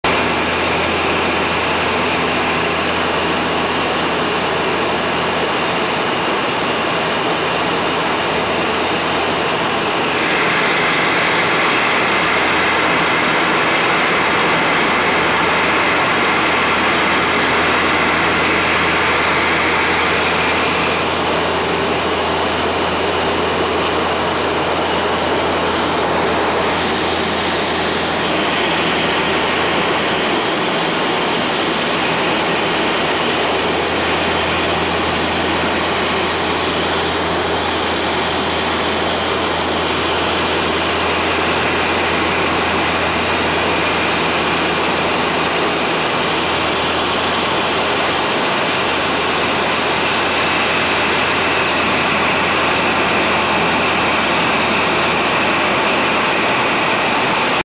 Low Pass over Frobisher Bay Fjords in a Twin Otter
Flying low over incredible Frobisher Bay Fjords in a Twin Otter on July 14, 2011.  Flight in a Twin Otter was chartered by the Canadian Society of Landscape Architects' annual conference in Iqaluit.